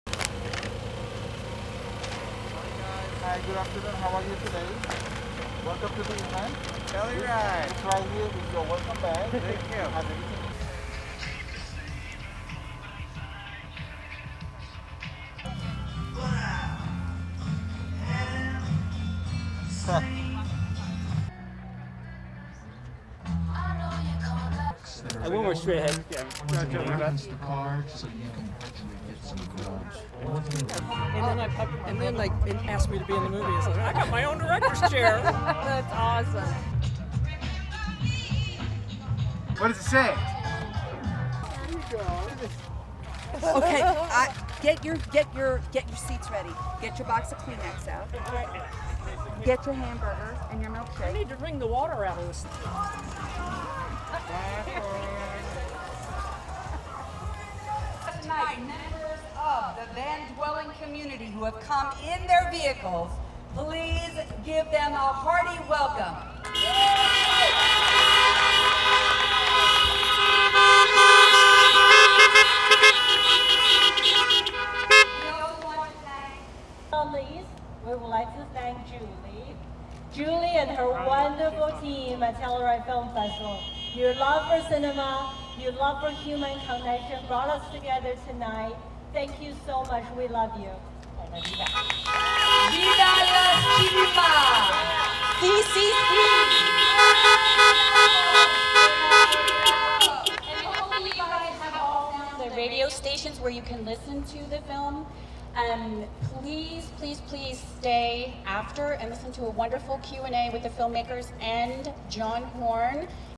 Nomadland Drive In Premiere - B-Roll and Behind the Scenes
Following the economic collapse of a company town in rural Nevada, Fern (Frances McDormand) packs her van and sets off on the road exploring a life outside of conventional society as a modern-day nomad. The third feature film from director Chloé Zhao, NOMADLAND features real nomads Linda May, Swankie and Bob Wells as Fern’s mentors and comrades in her exploration through the vast landscape of the American West.